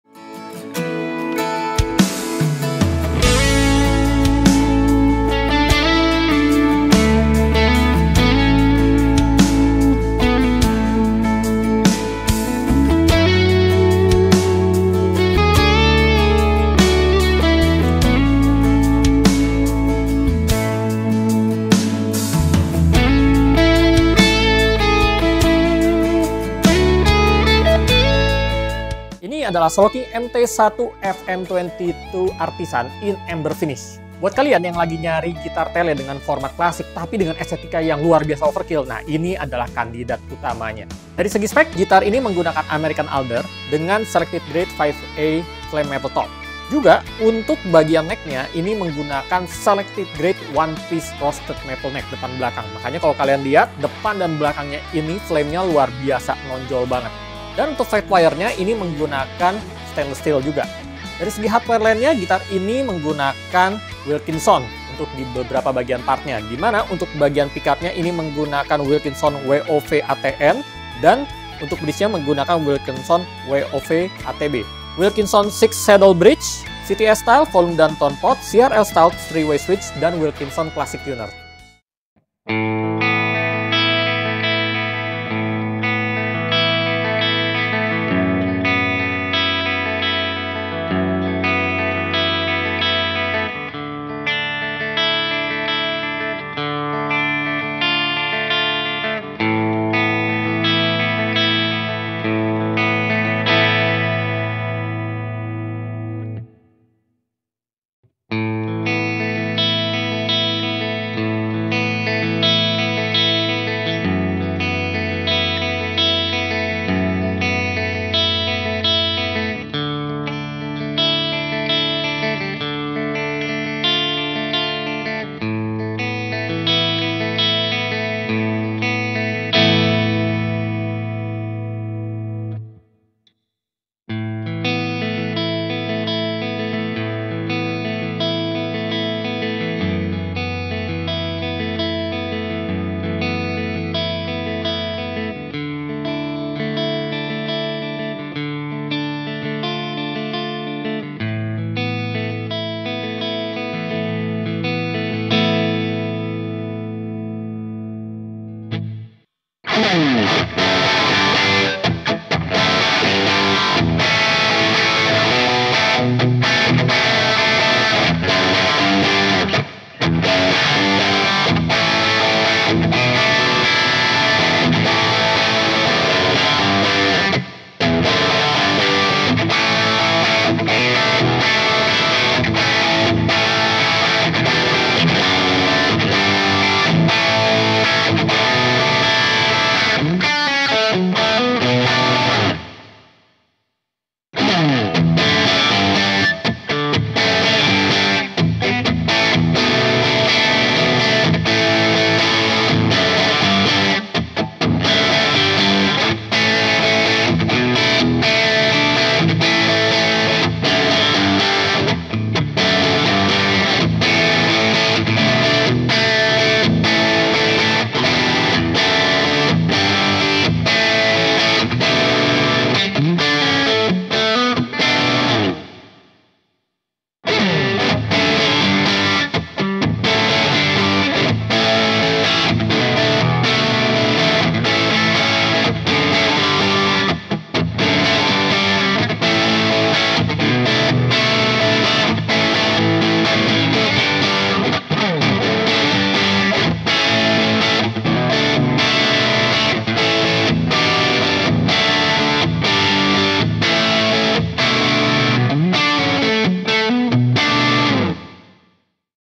SOLOKING MT-1 FM 22 ARTISAN FMN In Amber! Quick Sound Test
- American Alder body with 6mm Thick AAAAA Solid Flame Maple Top
- One Piece Artist Grade Canadian Roasted Flame Maple Neck with Modern D Shape and Glossy Finish
- Neck dimension 21mm (1st Fret) and 22.5mm (12th fret)
- 9.5 Fingerboard Radius
- Black Dot Inlay
- 42 mm Nut Width Nubone XB
- 22 Medium Jumbo Stainless Steel Frets
- Spoke Wheel Truss Rod Adjustment for easy access
- Volume and Tone Control Knurled Dome
- Three way switch CRL Style + CTS Style Pots
- Wilkinson Classic Deluxe Tuners
- Wilkinson Six Saddle Bridge
- Wilkinson WOVaTN Alnico V neck pickup and WOVaTB Alnico V Bridge Pickup
- Custom Run Exclusive to Nafiri Music